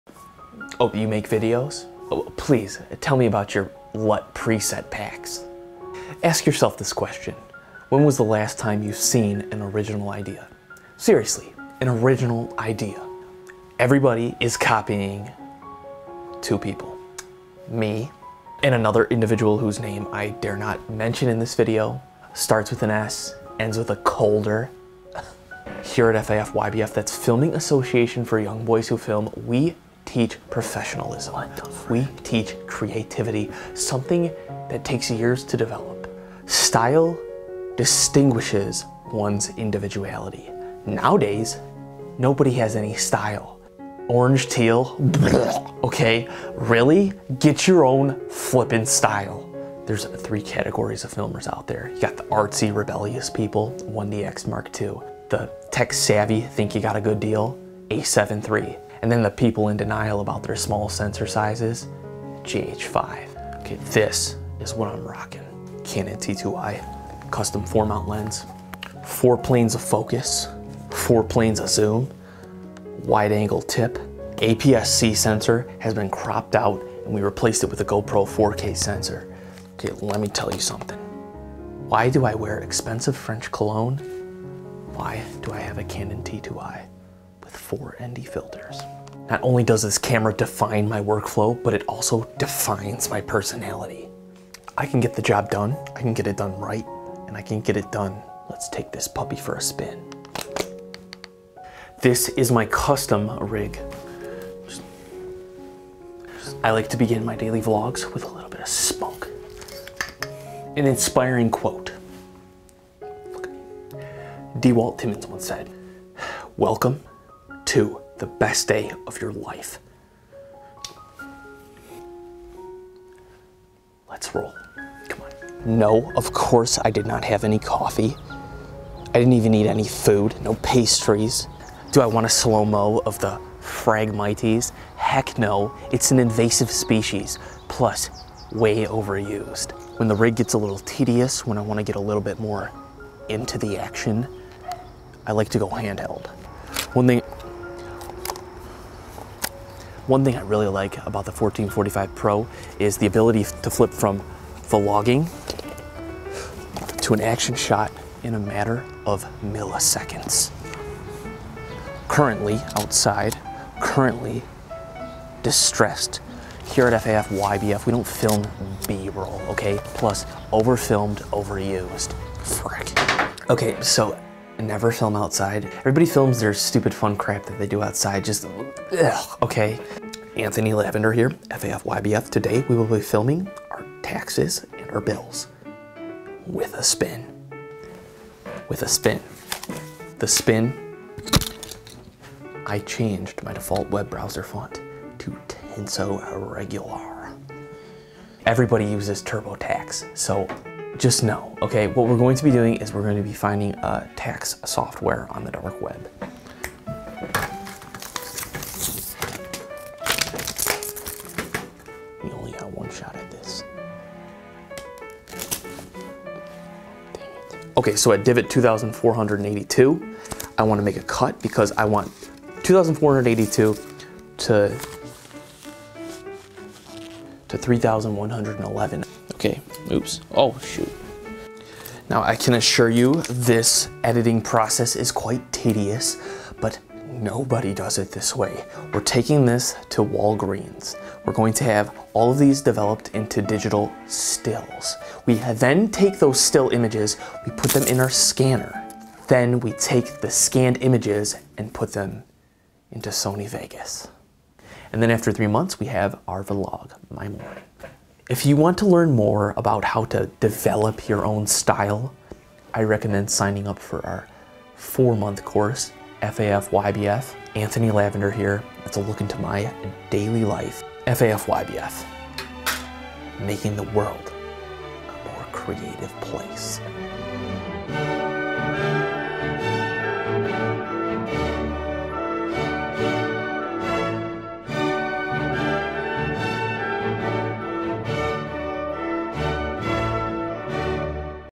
Strangest Camera In The World
Finally, someone with an original vlogging style. The most ridiculous camera ever. Nobody does it like this so it is not copying. Anthony at FAFYBF rants about the over done travel vlog style, the typical people who make videos, and then he shows you his perfect camera set up.

My Equipment:

[My Camera]
[Lens Used in This Video]

[Super Patrons]

Dave B.
Clark R.
Jamie M.
Killabees
Simon T.
Jonathan C.
Greg I.